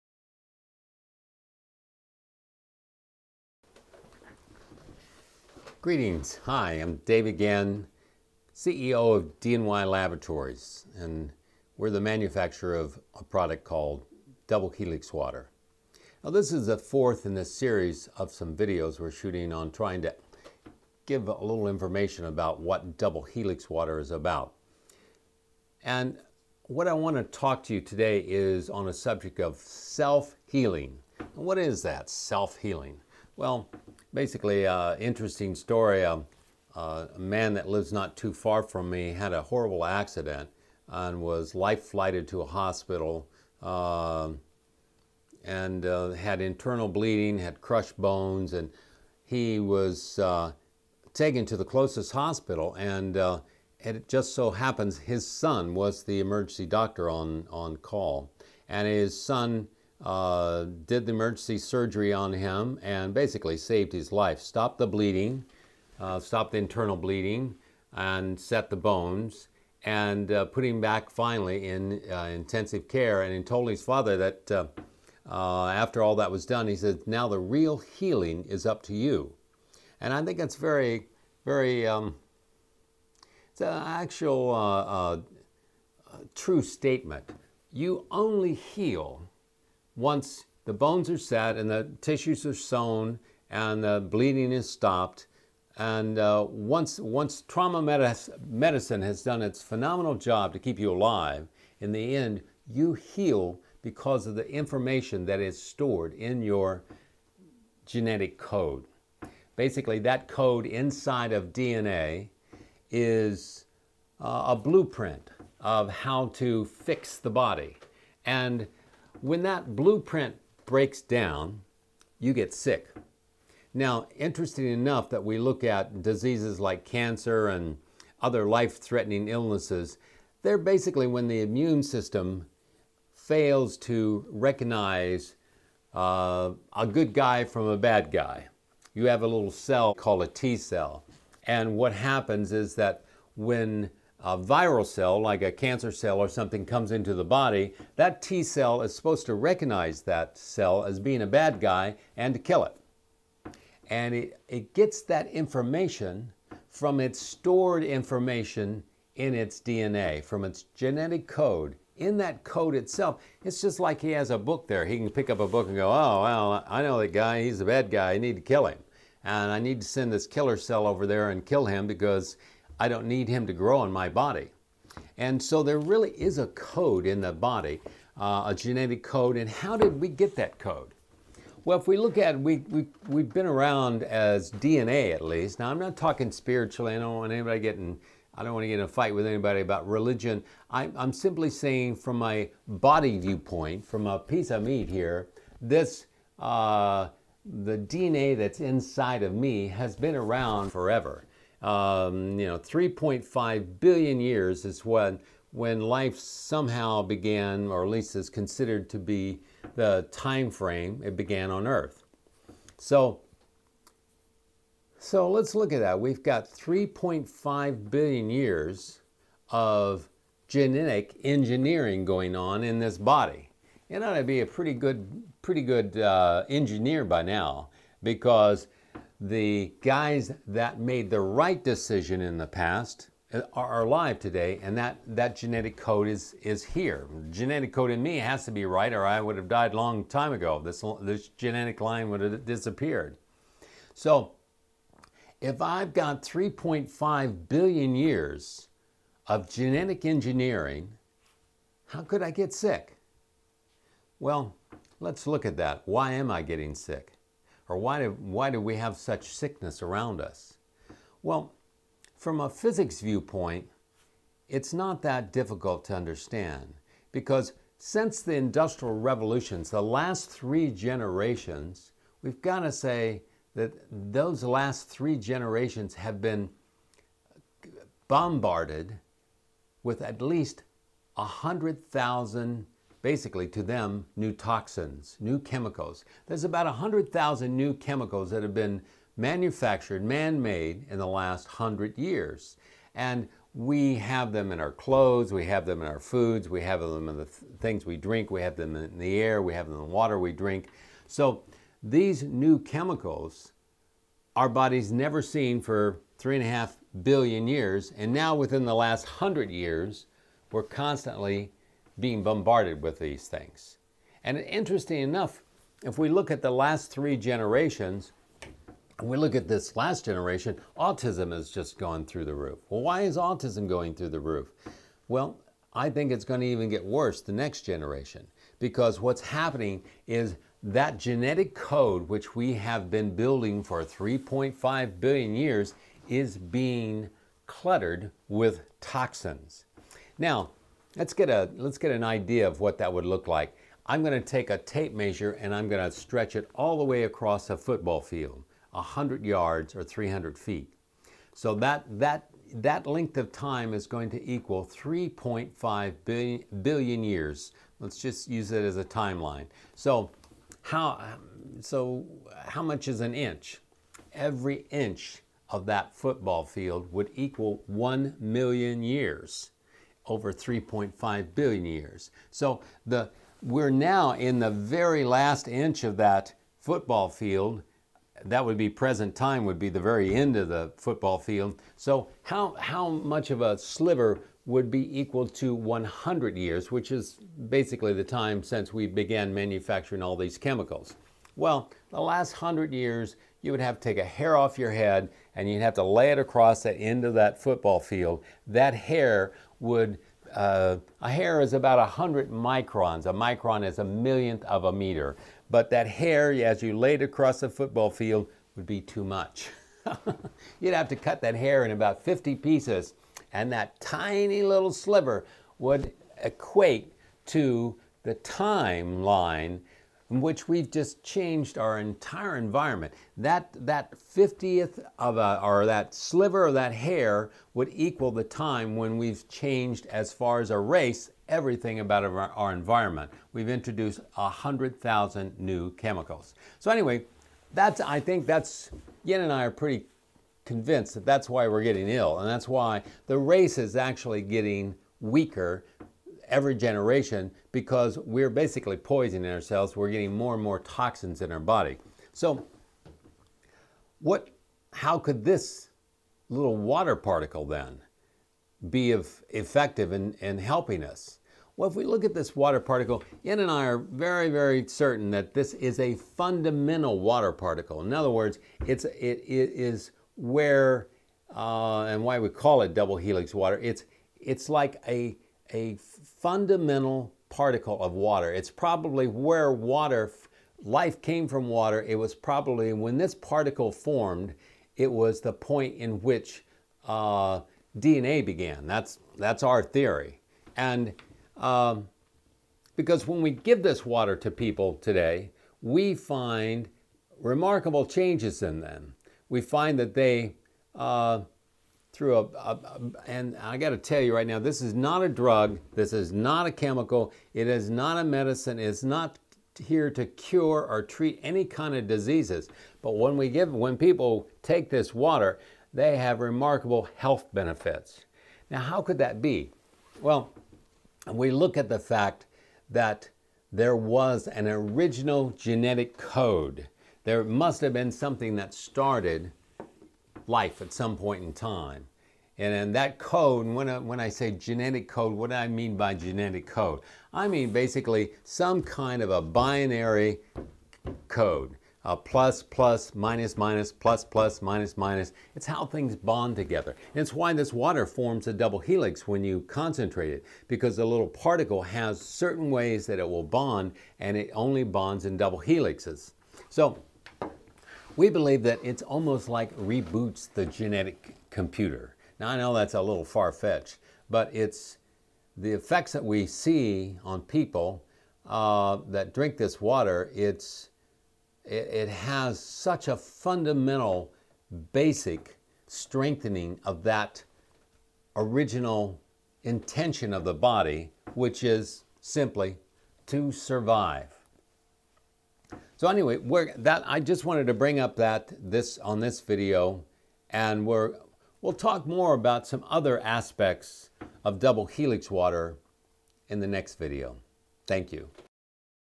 Self Healing, what is it, how does it relate to your DNA?
Self Healing, what is it? How is it related to the information stored in your DNA?

Subtitles available in several languages using Youtube's Closed Caption features.